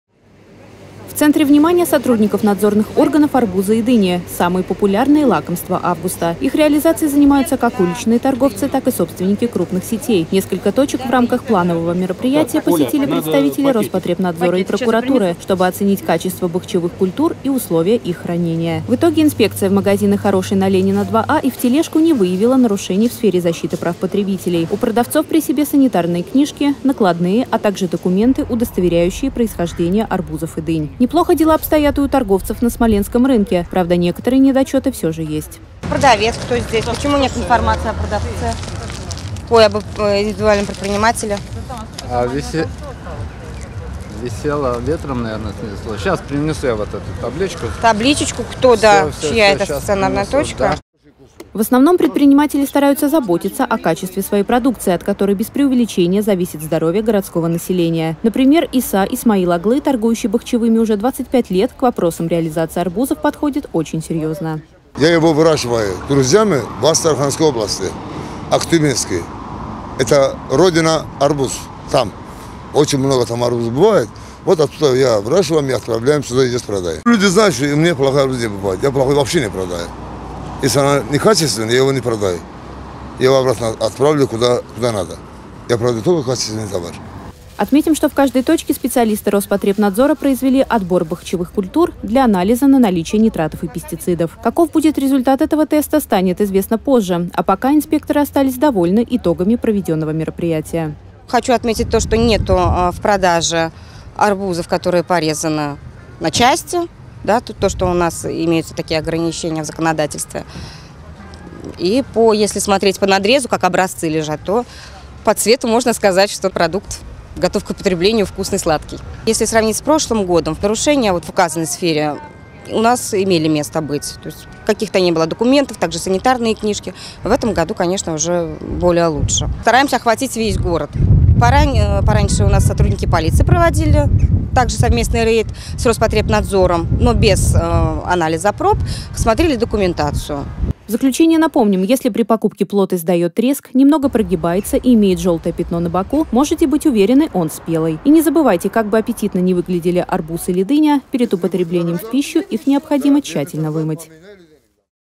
Сотрудники прокуратуры и Роспотребнадзора проверили места продажи бахчевых культур
На сегодняшний день особым спросом у великолучан пользуются бахчевые культуры, которые уже несколько недель украшают местные торговые прилавки. За качеством предлагаемой продукции, безусловно, следят сотрудники Роспотребнадзора. На этой неделе они вместе с представителями городской прокуратуры провели рейд по местам продажи  арбузов и дынь&nbsp;&rarr;